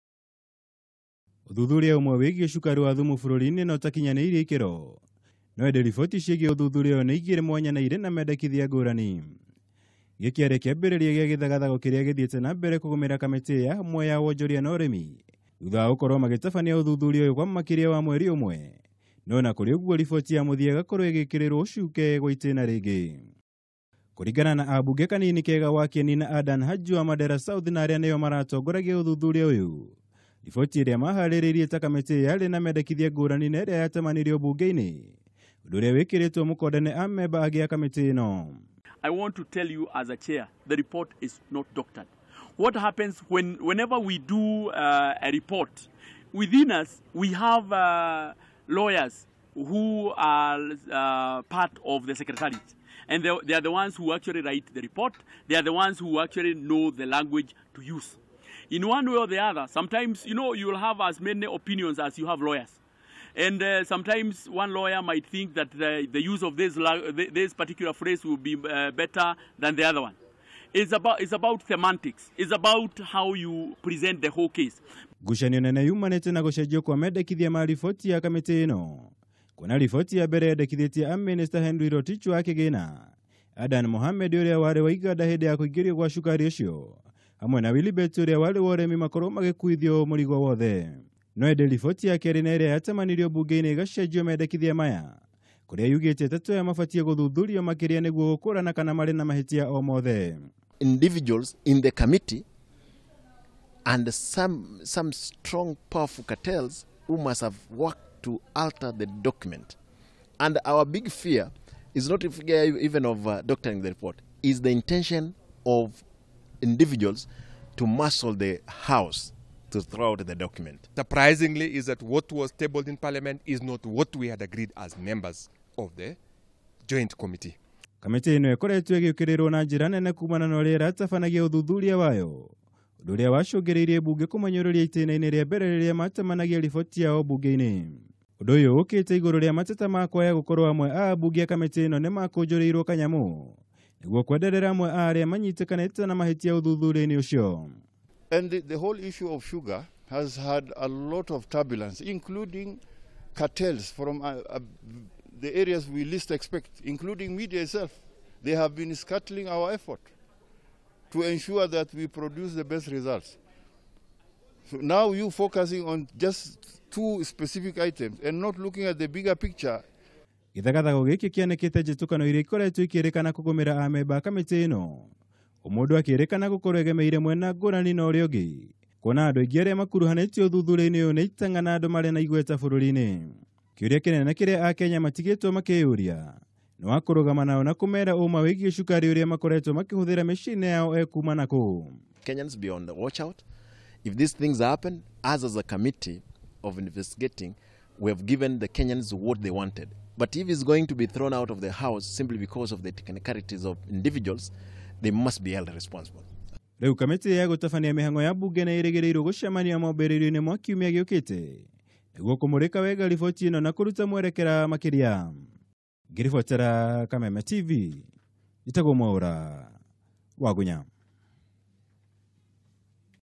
Ambunge matiganite kuuga riboti igii cukari iria mahaririirie niithogetwo
Githagathago kiria kigumiire uthuthuria wigii kuingirio gwa cukari utakinyaniirie ikiro guku bururi-ini nogithiite nambere na gwithegea, thutha wa aikariri giti a kamiti ya hamwe iria iratabanagia uthuthuria uyu hamwe na gikundi kia amember a kamiti iyo gwithemana na riboti iria iratemanirio mbunge kiumia giki  makiuga iyo ti riboti iria mahariirie ta kamiti kwona niithogetwo.
Uu ni ona thengia wa kwirikana ati kwaya gukorwo hena andu marakunjiirwo kanyamu niguo guthoga riboti iyo na iria irendekithitie gutacukirio makinya marumu kuri amwe a aria makuruhanitio na kuingirio gwa cukari ucio guku bururi-ini.